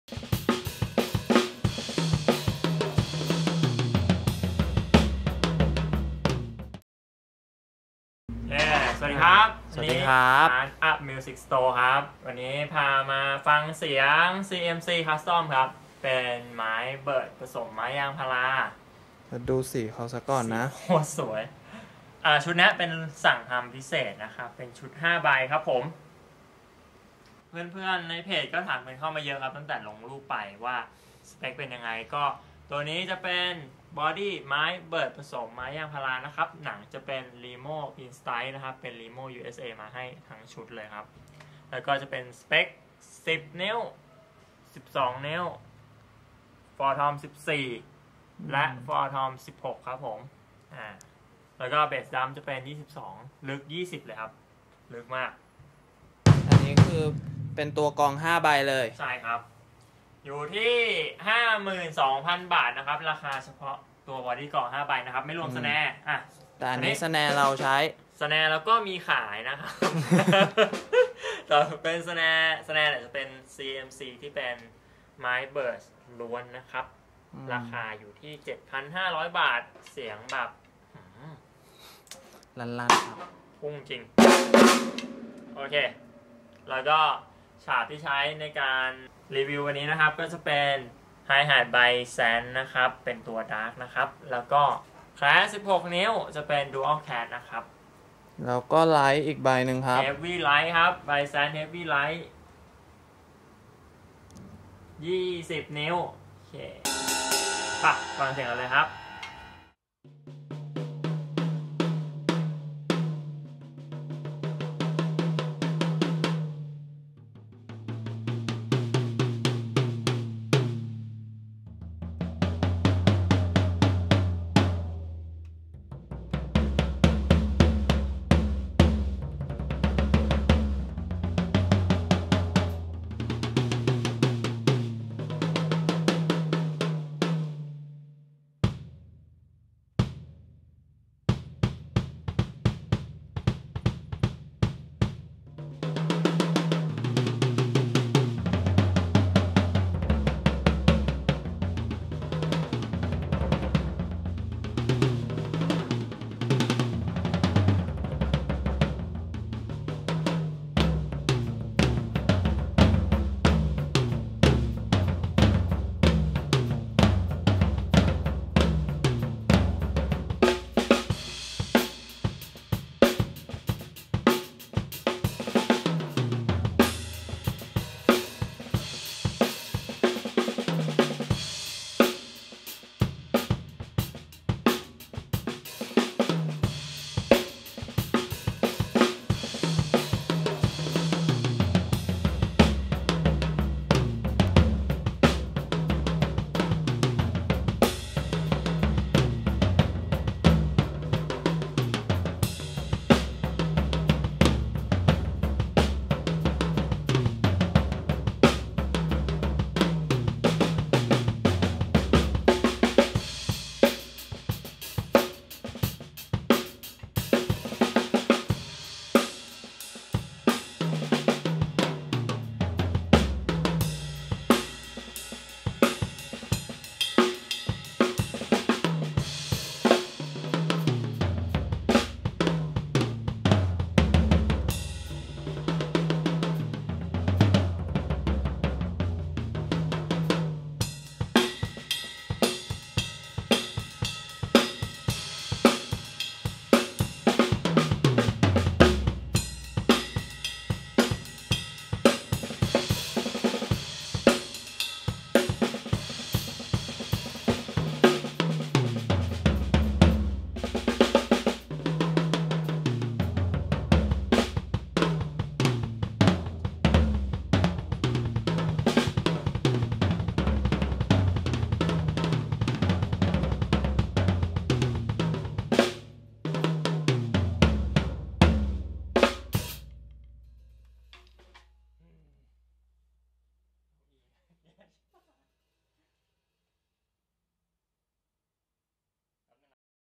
CMC Hybrid Custom - Aqua Blue Swirl Sparkle | UP MUSIC STORE
กลองชุด Custom ทำสีพิเศษจาก CMC มาดูความโหดกัน
ชุดนี้เป็นสเปคพิเศษนะครับ Shell เป็นไม้ Birch ผสมกับไม้ยางพารา จับคู่มากับหนังกลอง Remo USA รุ่น Pinstripes มาให้ทั้งชุด
- สำหรับชุดนี้ ให้เสียงที่มีทั้งความพุ่ง และความหนักแน่นอยู่ในตัว ที่เกิดจากการออกแบบ ผสมไม้ 2 ชนิดเข้าด้วยกัน ทำให้เกิดซาวด์ที่แตกต่างอย่างชัดเจนครับ
- ในส่วนของงานสี เป็นงานทำสีจริงลงบนตัวถัง โดยใช้ชื่อสีว่า Aqua Blue Swirl Sparkle ซึ่งมันสวยมากกกกกก !!
- ราคาเฉพาะตัวกลอง 5 ใบ อยู่ที่ 52,000 บาท ครับ
- 10x8"
- 12x9"
- 14x14"
- 16x16"
- 22x20"
- เขาแขวนทอม x 2
📬 สนใจสอบถามเพิ่มเติมได้ที่ร้าน อัพมิวสิคสโตร์ 🙏